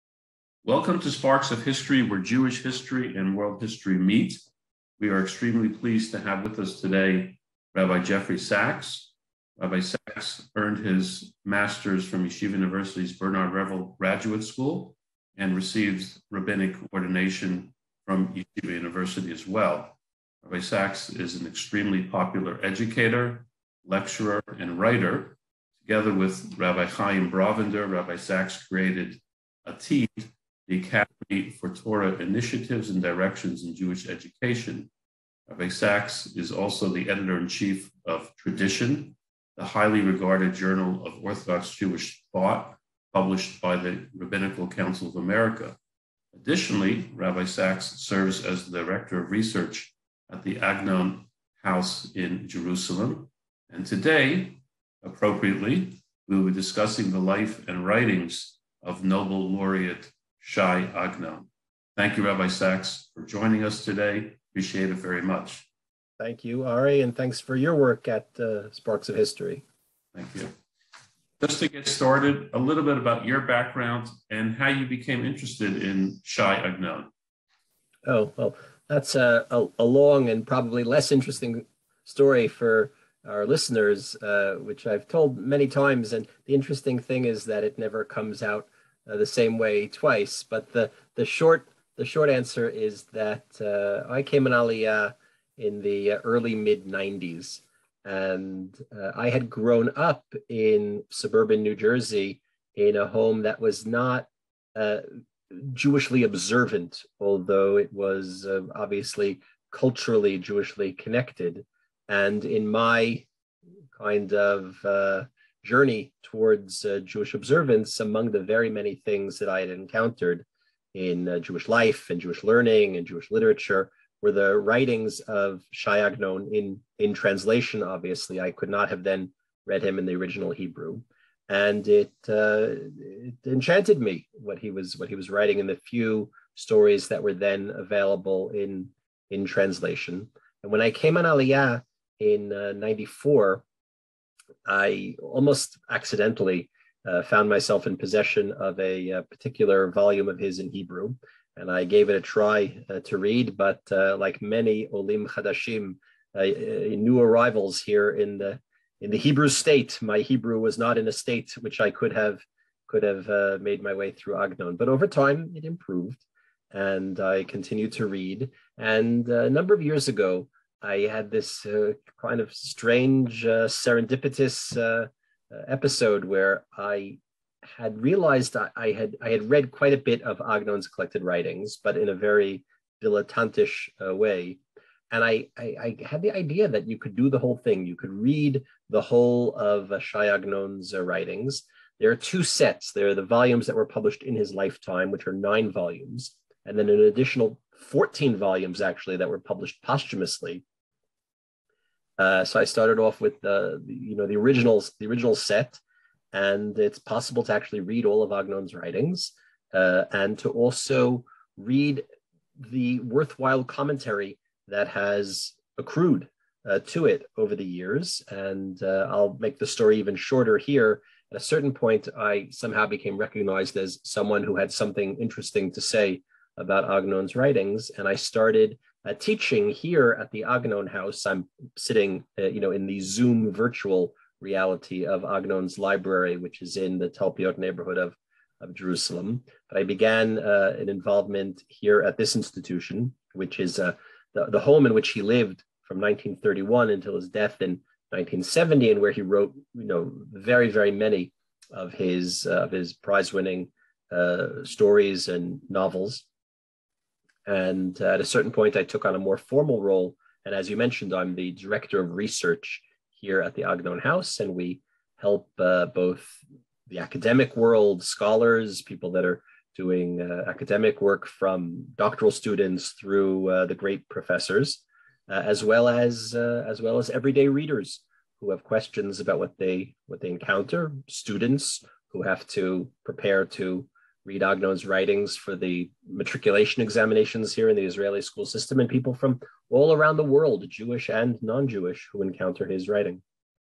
Shai Agnon – Rabbi Jeffrey Saks – Intro
Shai Agnon - Rabbi Jeffrey Saks

M.A. Bernard Revel Graduate School. Medieval Jewish History
Rabbinic Ordination Yeshiva Yitzhak Elchanan (RIETS)

Created ATID, Academy for Torah Initiatives & Directions
Launched WebYeshiva.org, first fully interactive online yeshiva

Leading expert on the writings of Shai Agnon
Director of Research at the Agnon House in Jerusalem.
Previous, Series Editor of the S.Y. Agnon Library, Toby Press

Editor in Chief, Tradition: A Journal of Orthodox Jewish Thought

#1 Shai Agnon – Rabbi Jeffrey Saks – Intro
Encountered Agnon in youth
Read English translated works
Began to read in Hebrew
Enchanted, goal to read all works
9 Volumes were published in Agnon’s lifetime
Another 14 posthumously
Head of Research Agnon House, Jerusalem